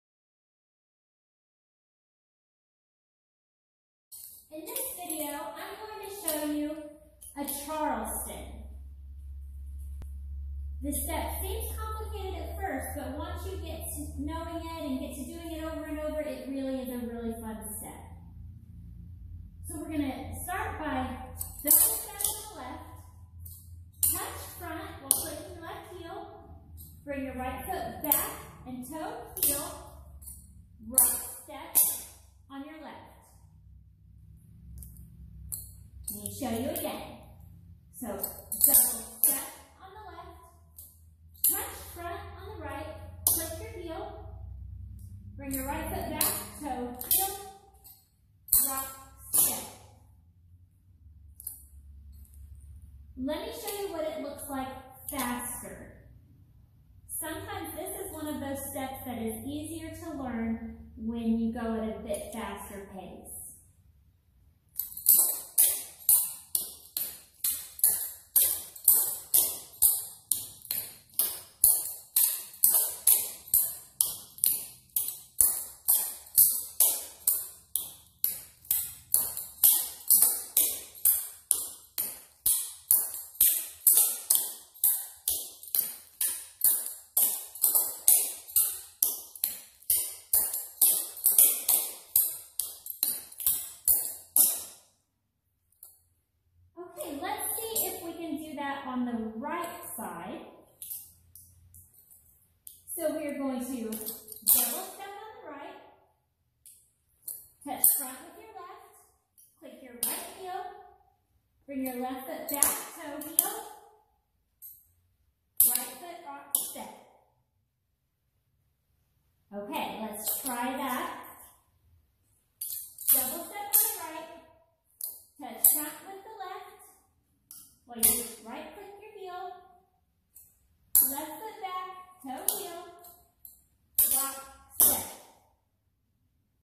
The Charleston Clogging Step (demo without music)
This video gives a demonstration of a clogging step called The Charleston.